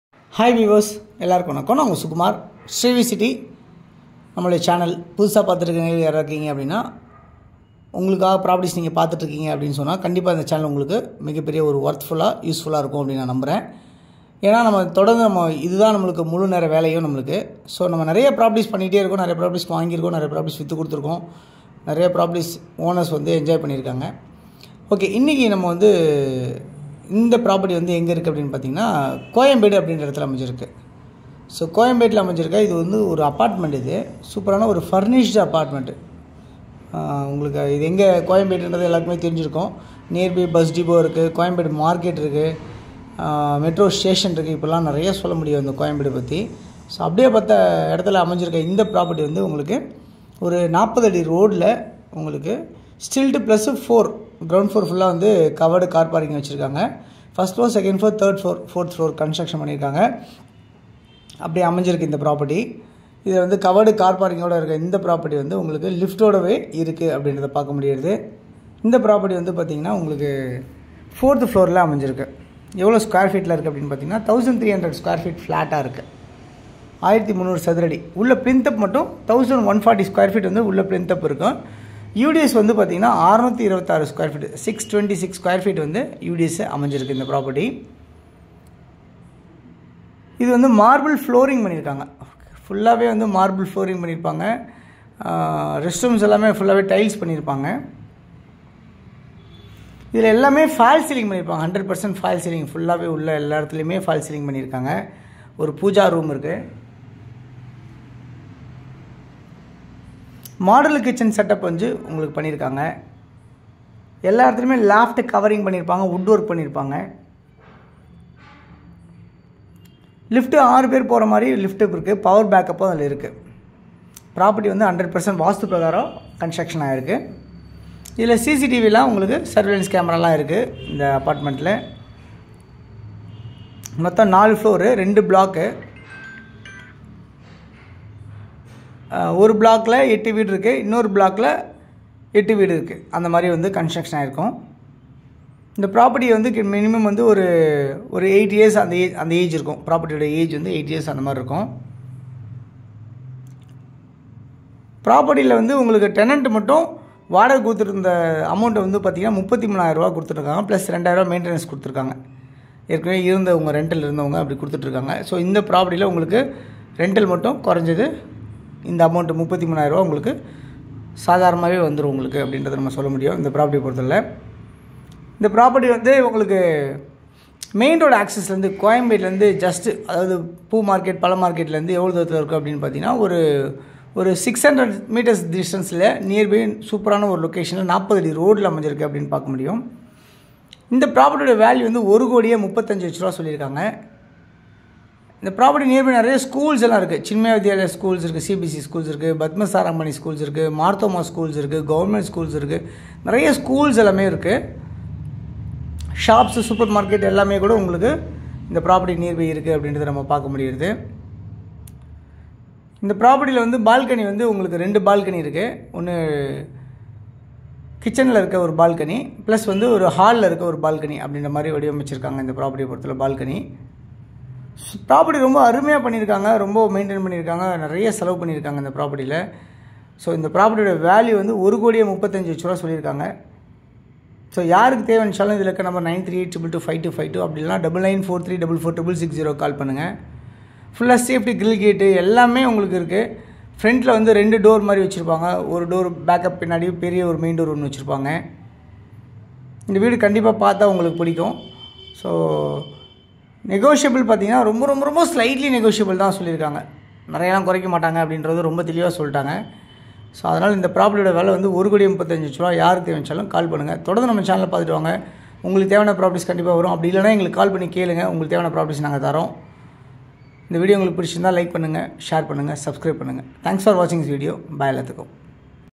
ID-1632, Hi-Fy Residential VIP Location Flats Sale In Koyambedu.
*3 BHK Furnished apartment for sale*

🔸Stilt + 4 Structure
🔸Reserved Car Parking(1CCP/Flat)
🔸4th Floor
🔸Marble flooring
🔸Saleable Area 1300Sqft
🔸Plinth Area 1140Sqft
🔸UDS 626 sqft
🔸100% False Ceiling
🔸Furnished along with Modular kitchen
🔸Johnson Lift 6pax
🔸Power backup
🔸100% Vasthu
🔸CCTV surveillance
🔸 Vacant Position
 rental getting 33,000 per month + maintenance charges(2,000)
🔸suitable for investors.
🔸selling price - 1.35cr.

CT.